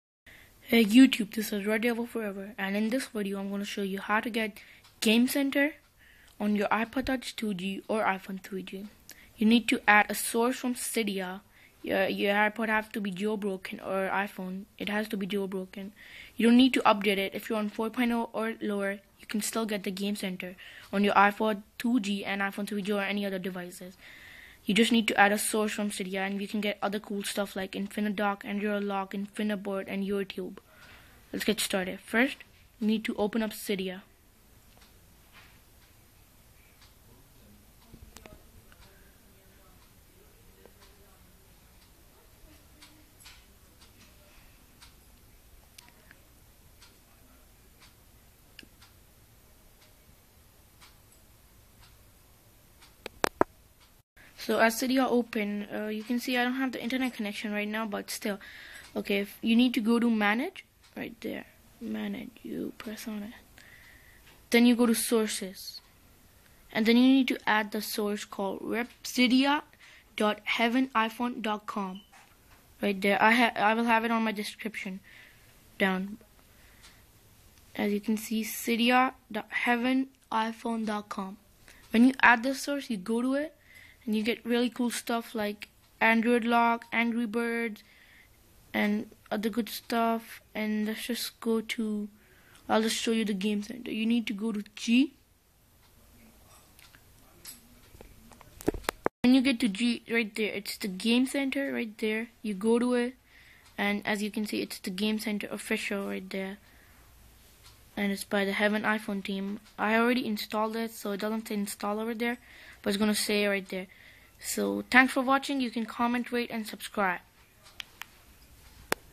How to get Gamecenter on ipod touch 2g or iphone 3g
This video will show how to get gamecenter on ipod touch 2g or iphone 3g without updating just need to add a source from cydia